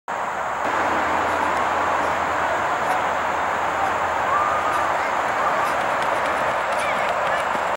Lynix Branch 40 yard run
Lynix Branch High School Sr.  Our Lady Of Mercy Catholic High School  DL/OL  40yd. Dash  Time= 5.19